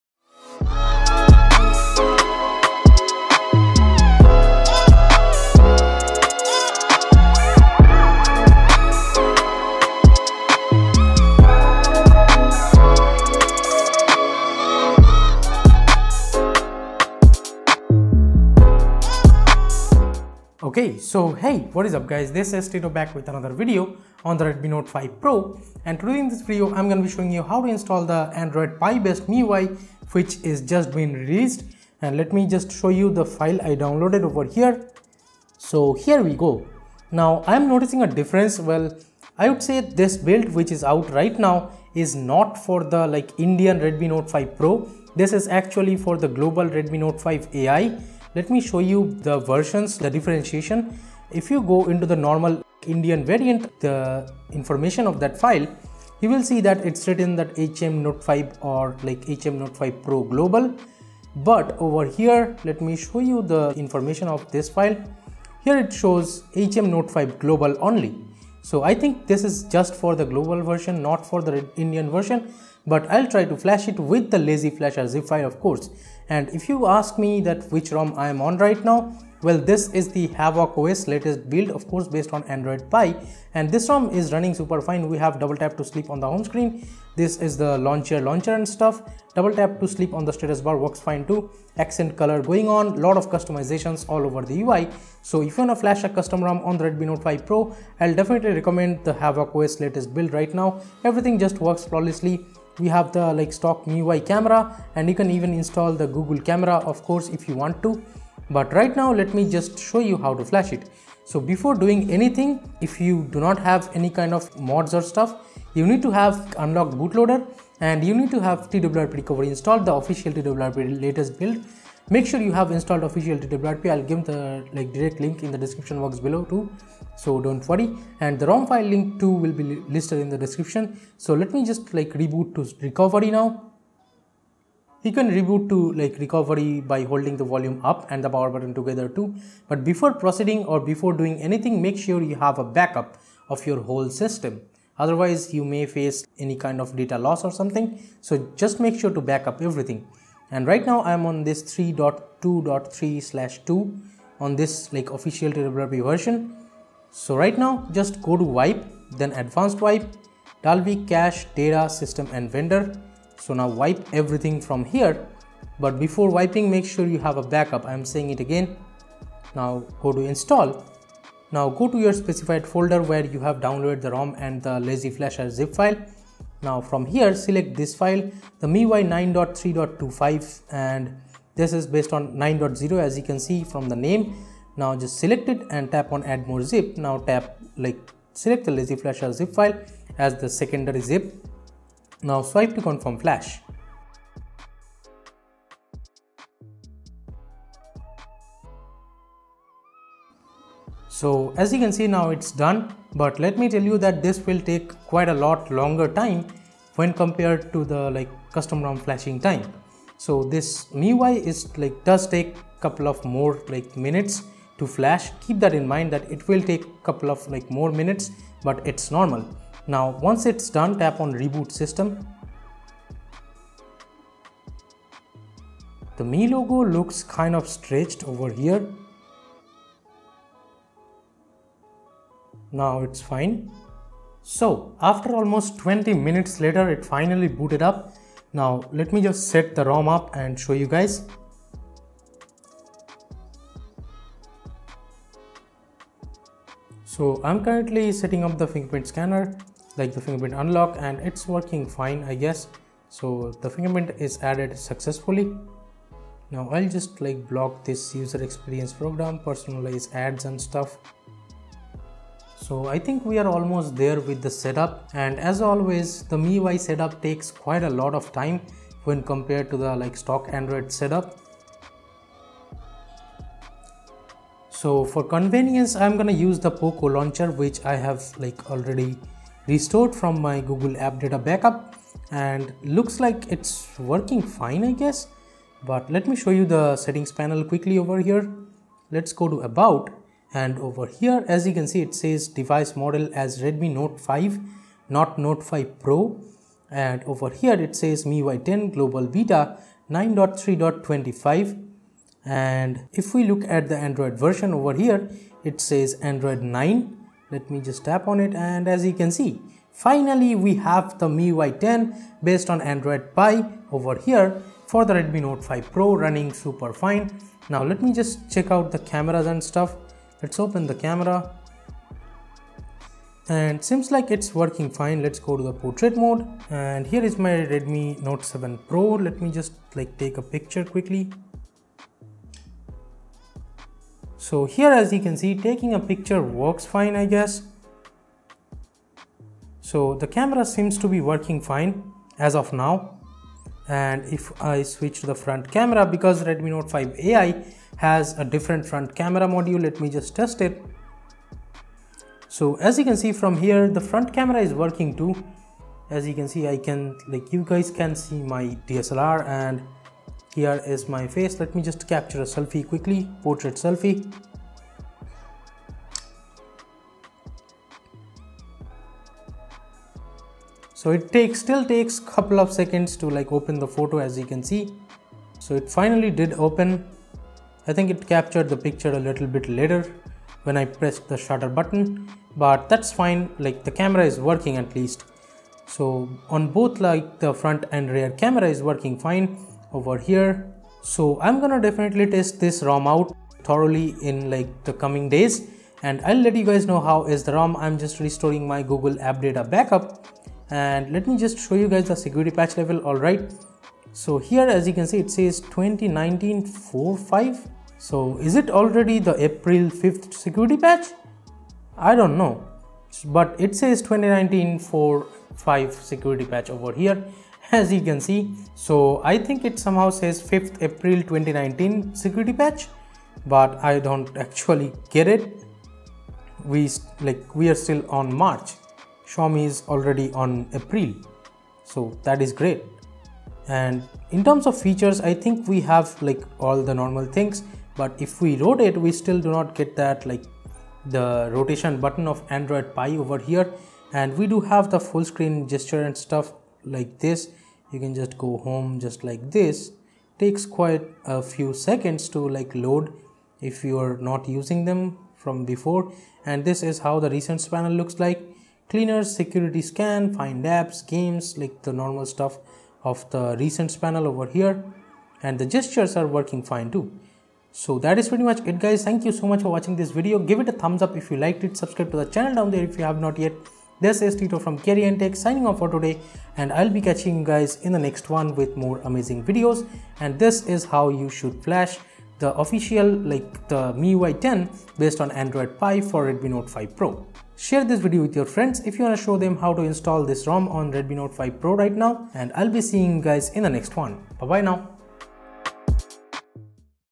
How To Flash MIUI 10 Pie 9.0 On Redmi Note 5 Pro (With TWRP) *DON'T FLASH*
*WARNING*
After installing with this method your TWRP will get encrypted everytime you boot back to the system. So I won't recommend flashing this rom if you don't want to get stuck with miui 10 on Pie.
Because you have to format data again almost every time you reboot to system!

Video Gear:
Canon 200D
Boya M1
Zoom H1n

Video Lighting:
Osaka OS 300 Mark III
Yongnuo YN 300 Air